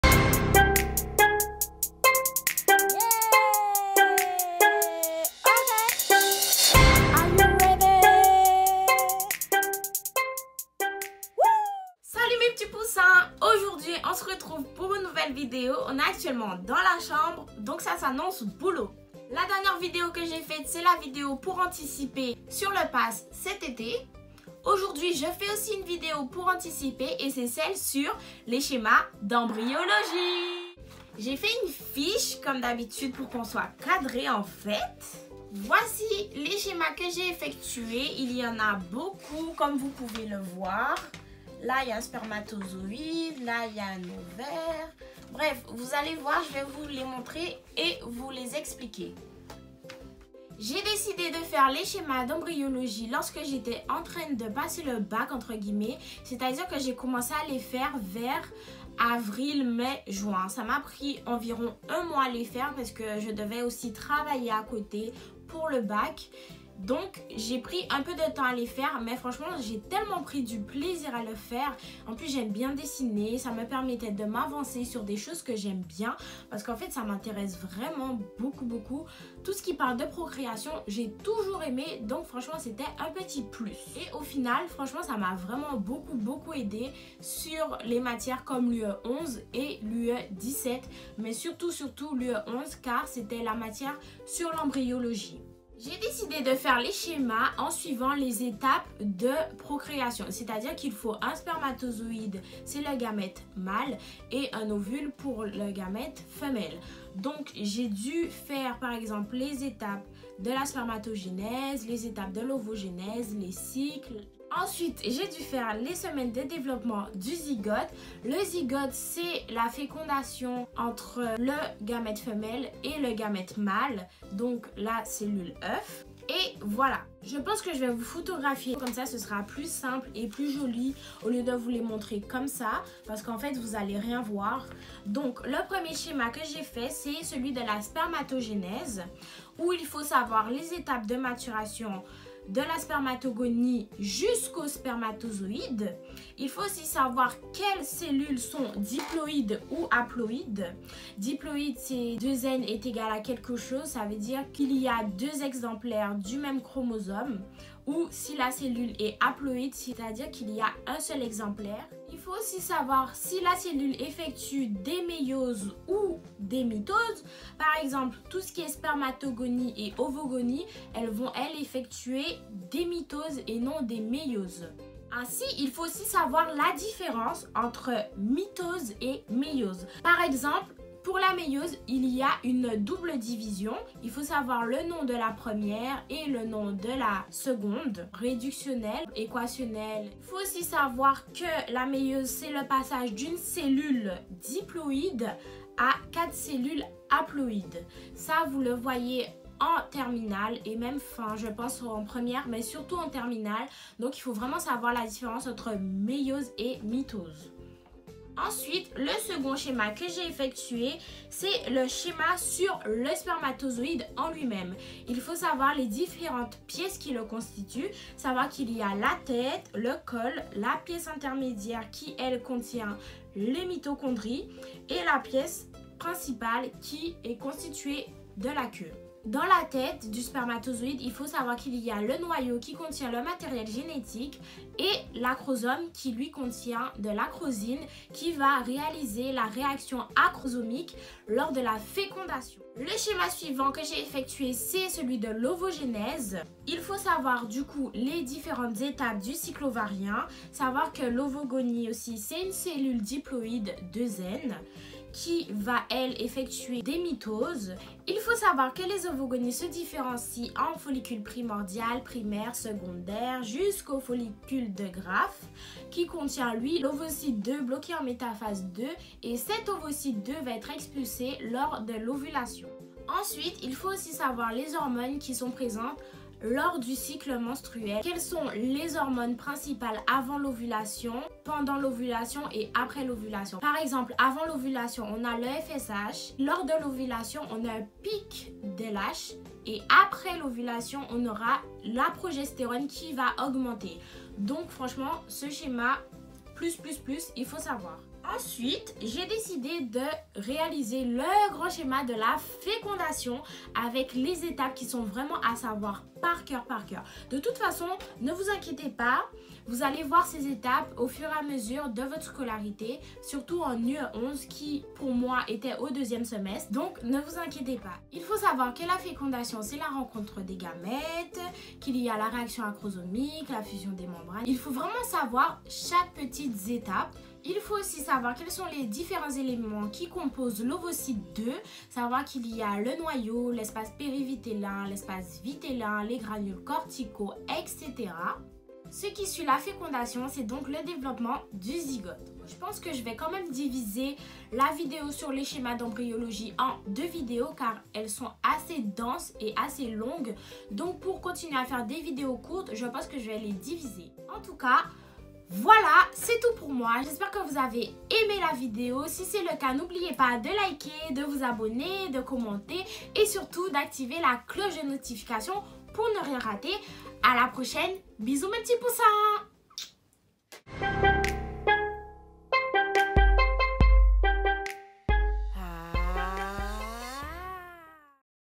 Les schémas d’embryologie pour anticiper ? PARTIE 1_ PASS/LAS 👶🏽
Salut, j'espère que vous allez bien !

Site des schémas pour une meilleure qualité : embryology.ch ❤️

Voici la vidéo que vous m’avez demandé ✨
Donnez tout et soyez fière de vous car le travail paye toujours !

N'oubliez pas de mettre la vidéo en 1080p